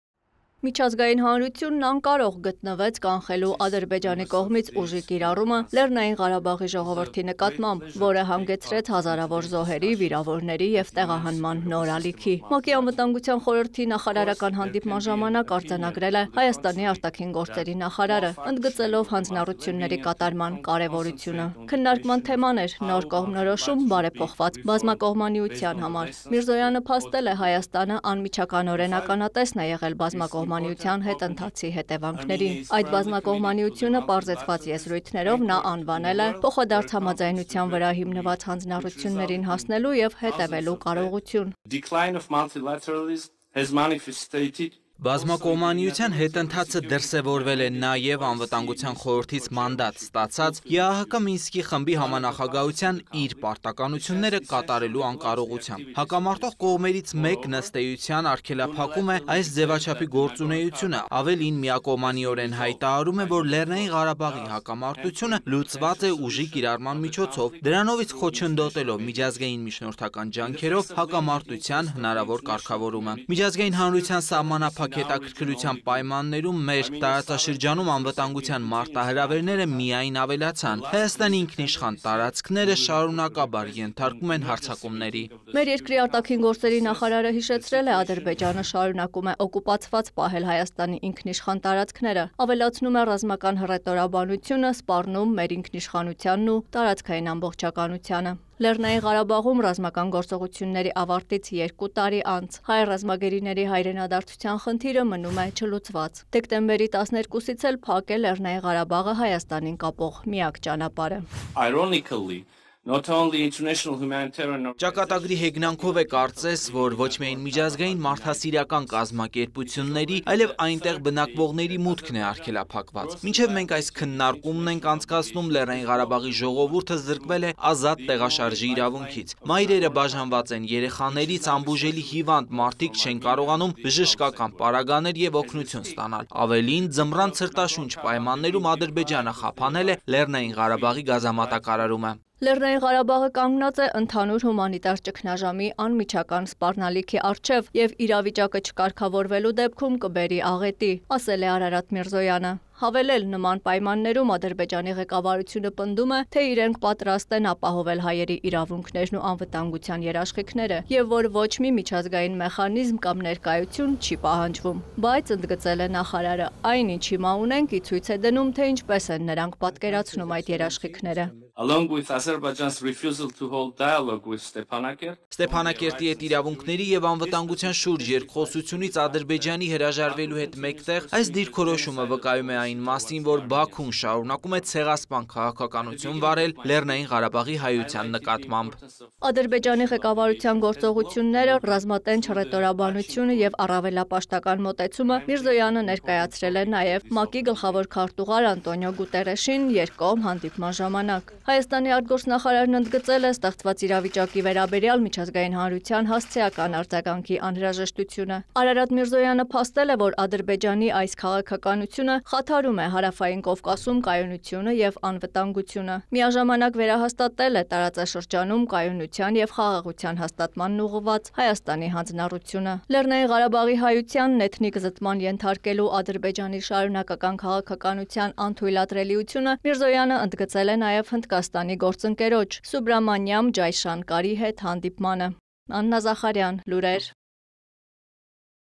Առաջին ալիքի անդրադարձը ՀՀ ԱԳ նախարար Արարատ Միրզոյանի՝ ՄԱԿ ԱԽ հանդիպմանն ունեցած ելույթին
Դեկտեմբերի 14, 2022թ.